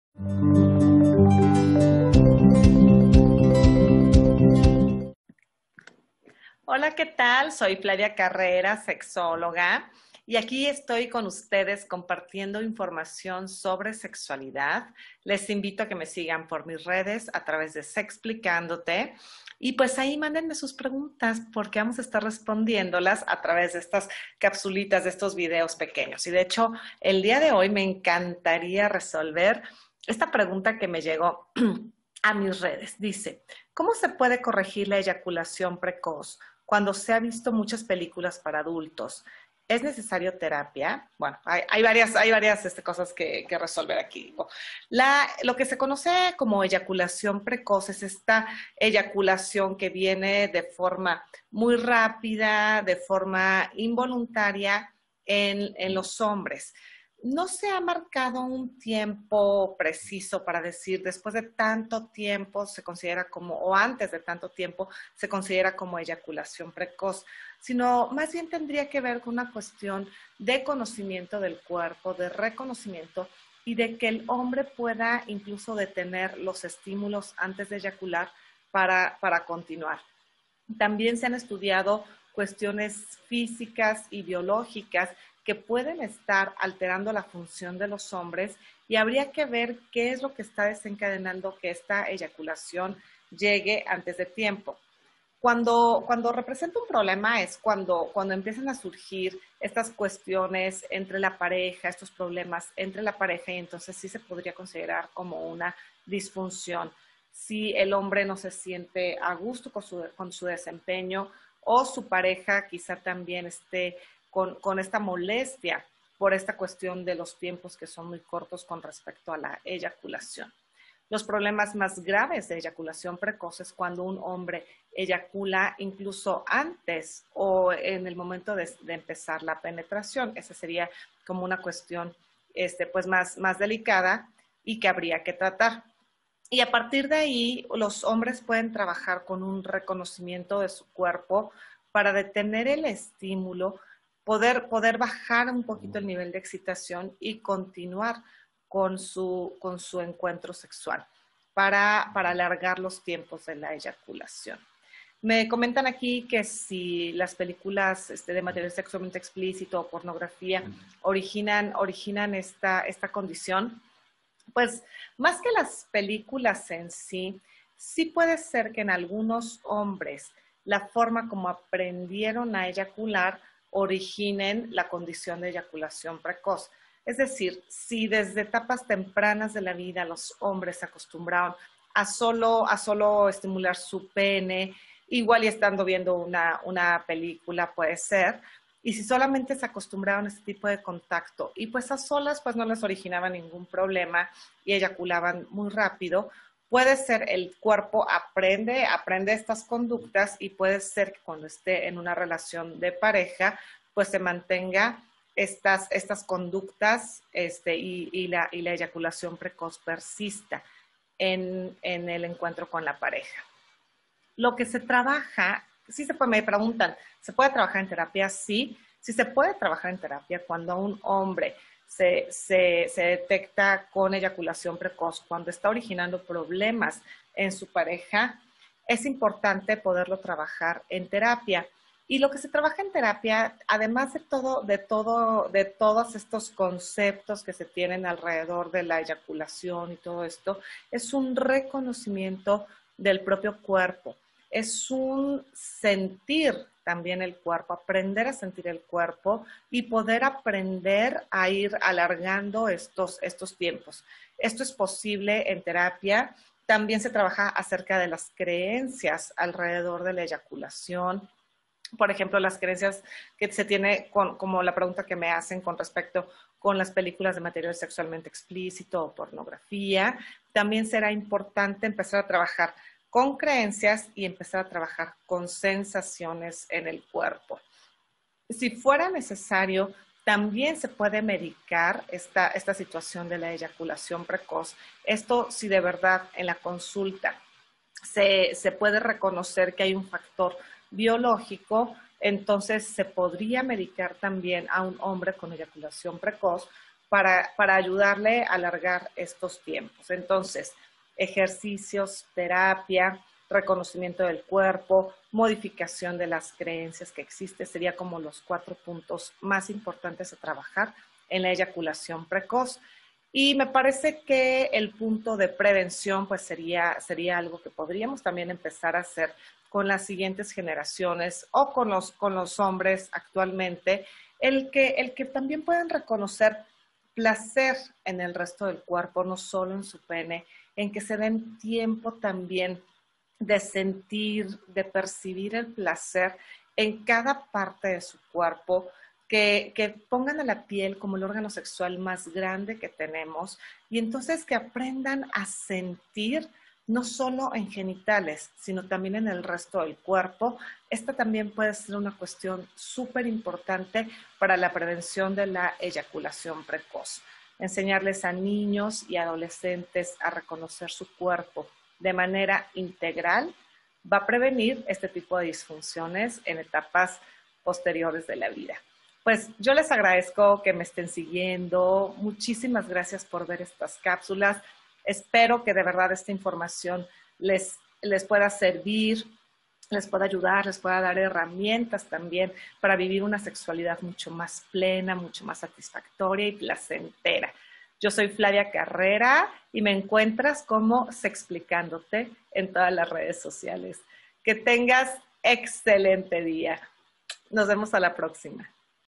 Eyaculación precoz
Quizá percibas que tu eyaculación llega antes de lo que desearías, o te gustaría que tu pareja durara más.  La eyaculación precoz es una disfunción común de la vida erótica, en esta cápsula platico sobre ella, te invito a verla.